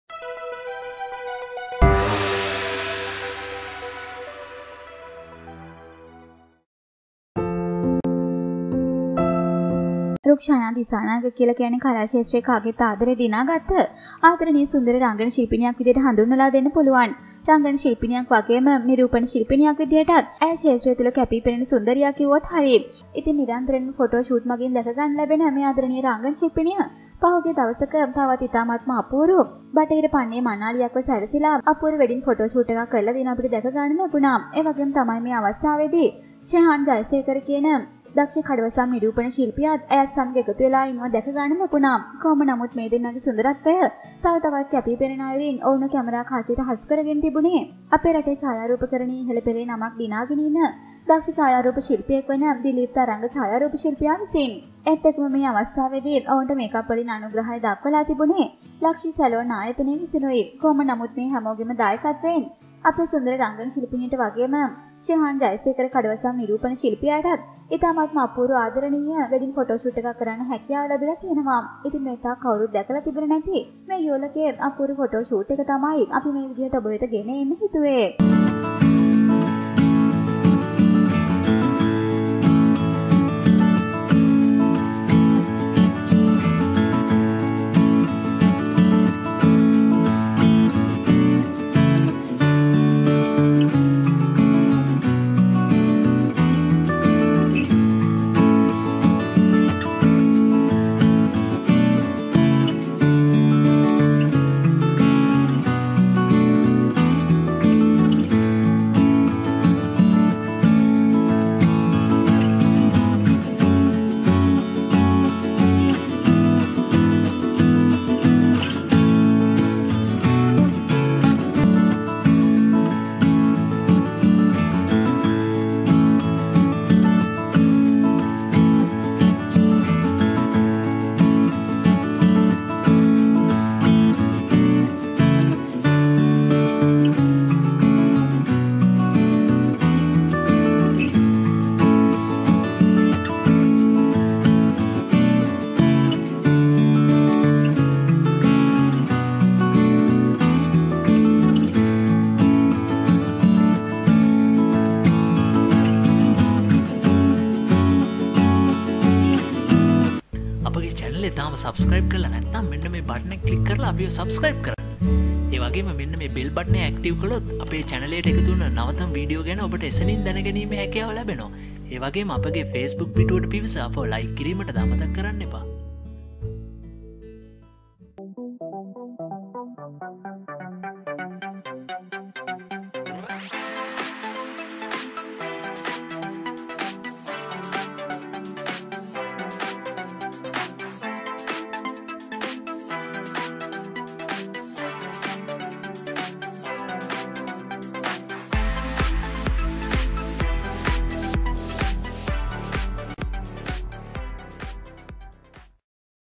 රංගන ශිල්පිනි රුක්ෂානා දිසානායක හිමකුමාරියක් වගේ සැරසිලා කරපු Photo shoot එක Rukshana Disanayaka
ඒ ටික නම් පට්ටයි නේද?
සුන්දර රංගන ශිල්පිනි රුක්ෂානා දිසානායක
හිම කුමාරියක් වගේ සැරසිලා දෙවෙනි මනමාලියන් වට කරගෙන කරපු Photo shoot එක  මෙන්න.Rukshana Disanayaka & Shehan Jayasekara
Makeup & Hair Style By - Lakshi Kadawathaarachchi LakshiSalon
Costume - LakshiSalon
Groom costume - grooms art
Groom hair style - Ruwan Darshana
Dilip Tharanga  Photography
Retouch- Pasindu Rathnayake
Videography - Charuka Priyankara
SPEACIAL THANKS TO Blossoms LakshiSalon